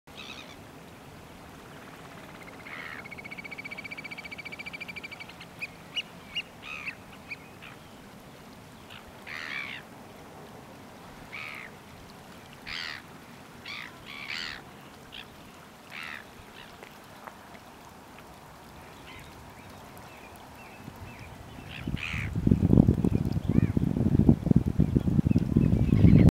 Leucristic Variable Oystercatcher at Duder's Beach
On the 23rd October 2020 I took this video of a leucristic oystercatcher at Duder's Beach, Clevedon., New Zealand. This species is usually black. Leucism is a term used to describe a wide variety of conditions which result in the partial loss of pigmentation in a bird—which causes white, pale, or patchy coloration of the feathers but not the eyes.